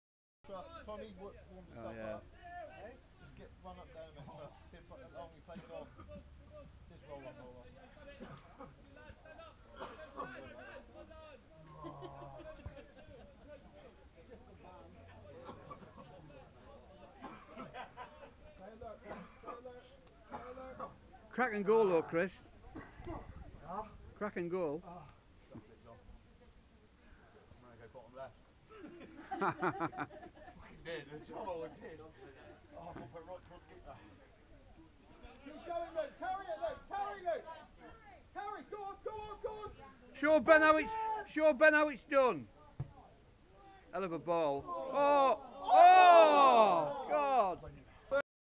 EAST CORNWALL League Cup Qtr Final, St Stephen 1st Team 3 v 2 Plymstock Res AET.(2-2FT)
Luke Damerell races out of defence with the ball and sends a great ball into Craig Coad who shoots wide of the near post.